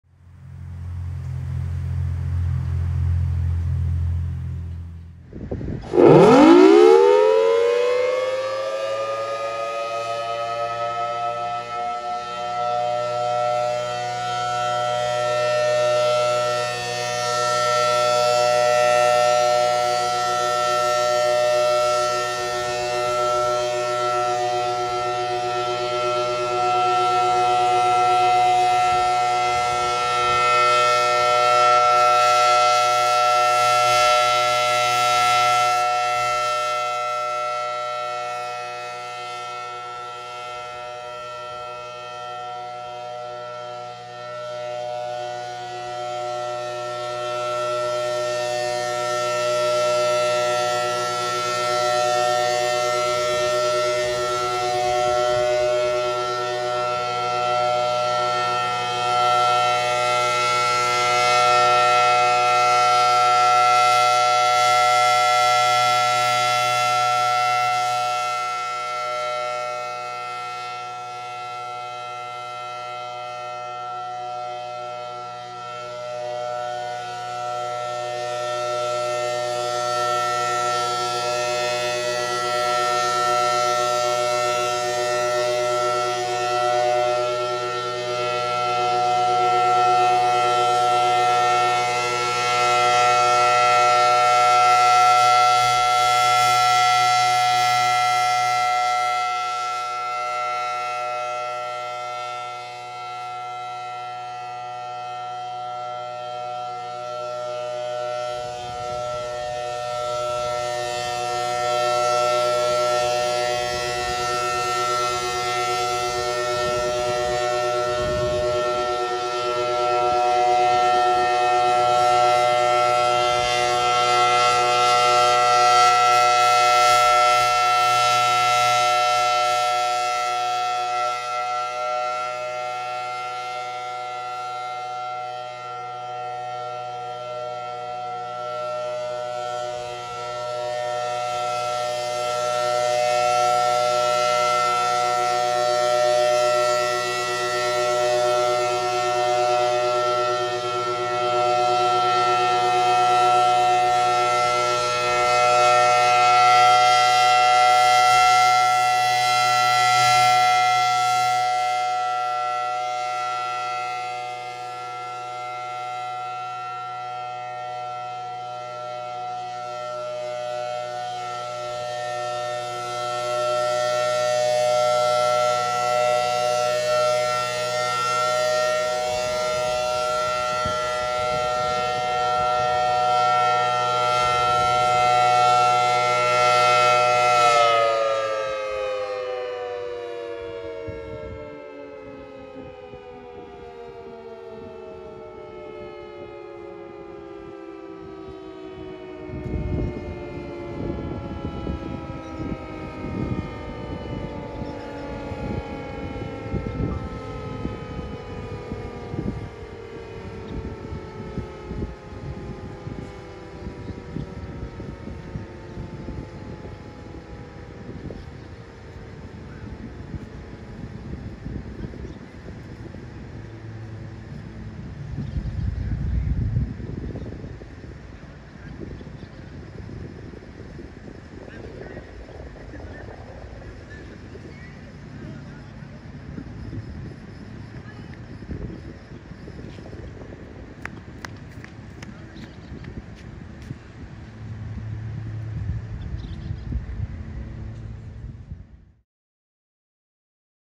FS Thunderbolt 1000AT | 3 Minute Alert | Lindsay, OK ( Garvin Co. Siren Test )
I got to meet up with Dillon Carpenter, OWS Crazy1, Sirens Of The Southwest, and JA Sirens! This was supposed to do attack, but I guess this time it was alert.

   At 12:06 PM, the City of Lindsay conducted a test of their outdoor warning sirens on Friday, July 17, 2020. The test lasted for 3 minutes and was pretty loud!

Afterwards we all ate at Boomerang Diner. It was good.

Siren Location: W. Creek Street and SW. 3rd Street.

Temperature:    91°F

Units Heard: Thunderbolt 1000AT ( 65 ft. )
                       Sentry 16V1T-B ( 813 ft. )

When Lindsay Tests: Every Friday at 12:00 PM ( Weather Permitting )